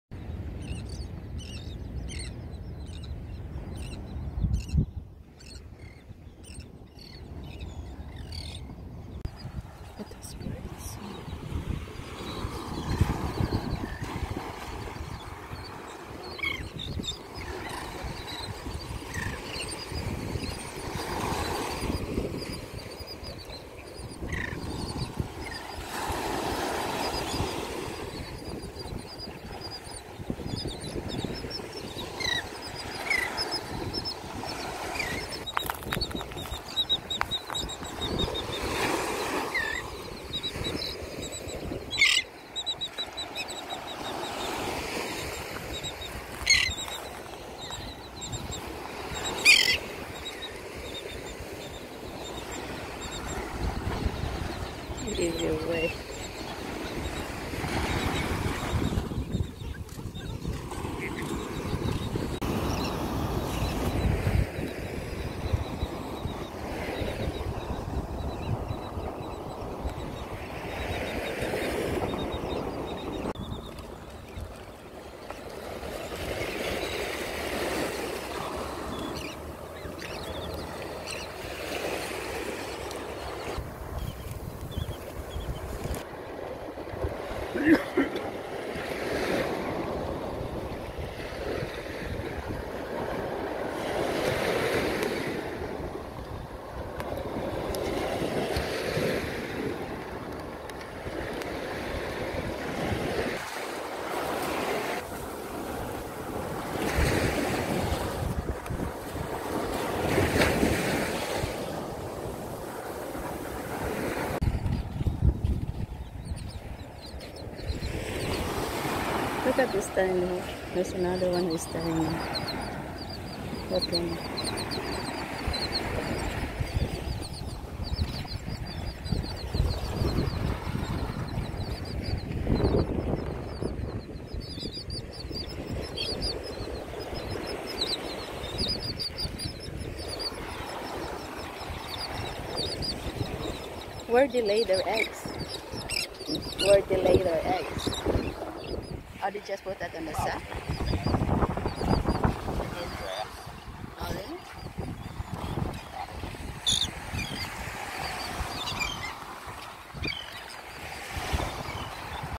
A Flock of seagulls at the beach/ Nokomis beach Florida
It's amazing to see these seagulls at the beach.